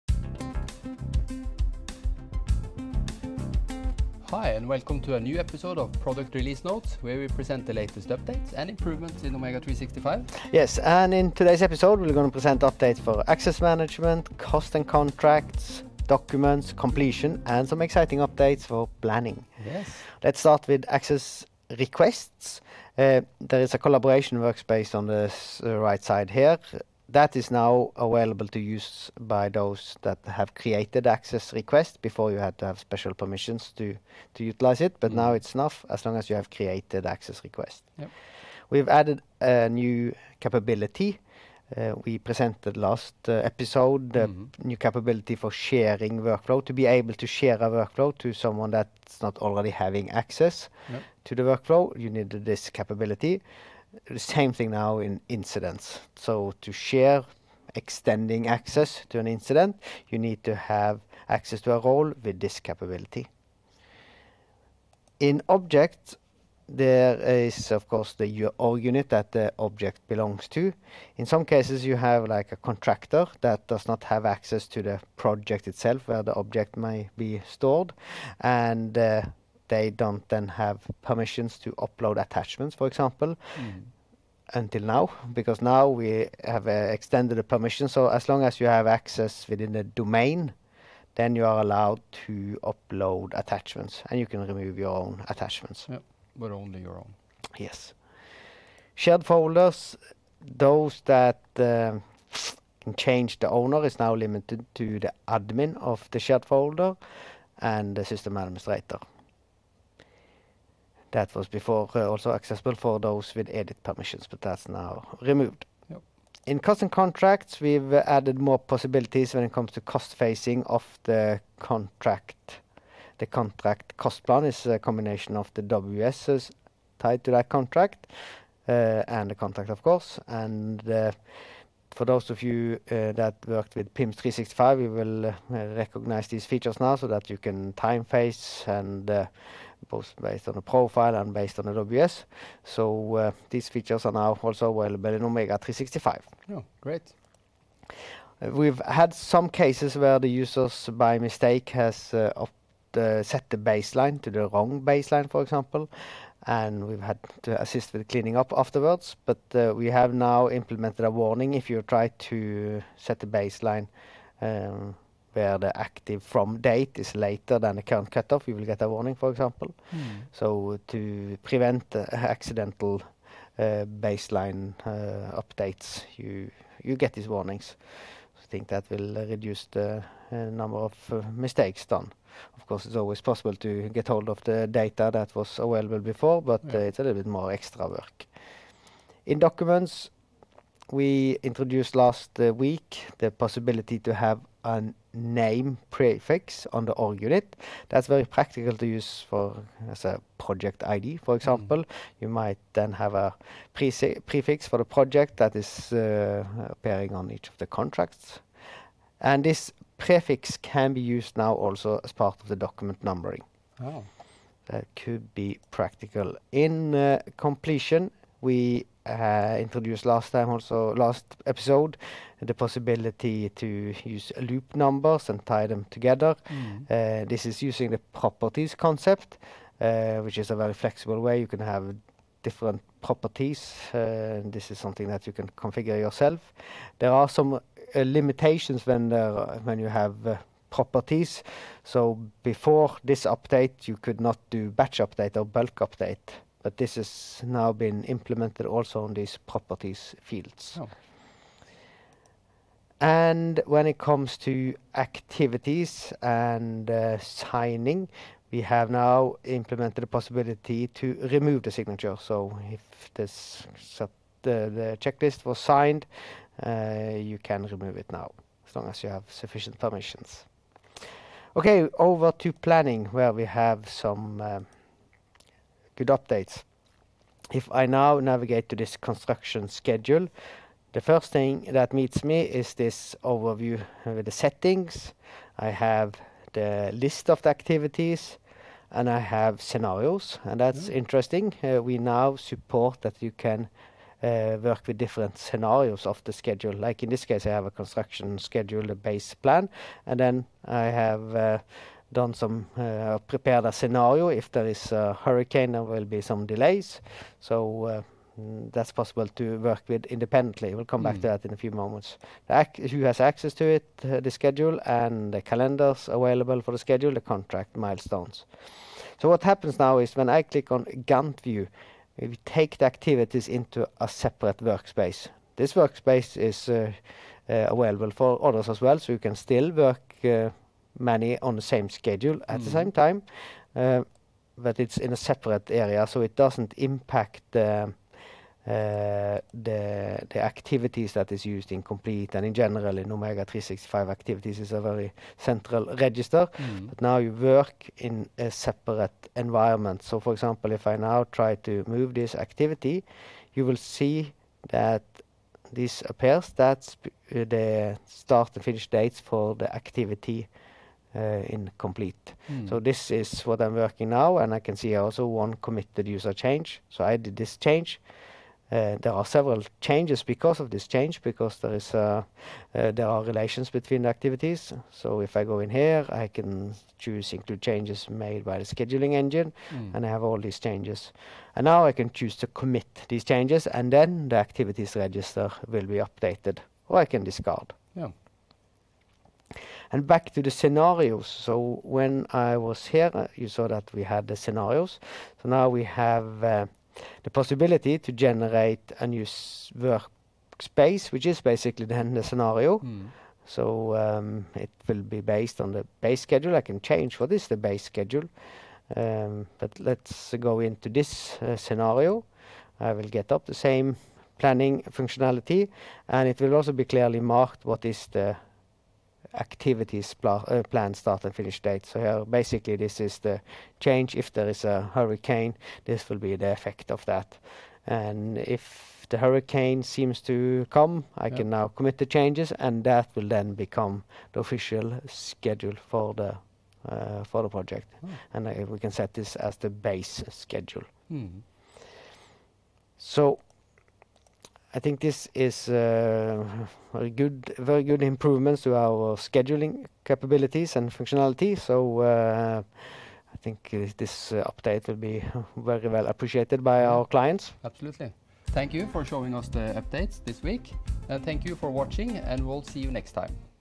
Product Release Notes 23 November (EN)
Get to know the latest updates in Omega 365. This short video gives you a quick update of the latest improvements which enhances your use of the system!

00:00 Intro
00:04 Agenda
00:20 Access Management
01:56 Cost and Contract
03:09 Document
03:37 Complete
04:32 Planning
07:38 Recap